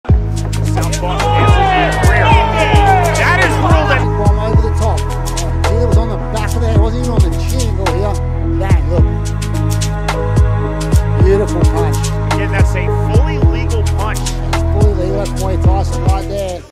Knockdown Lion Fight 65 - Jefferson Silva vs Ian Greer
Knockdown
Lion Fight 65 - Jefferson Silva vs Ian Greer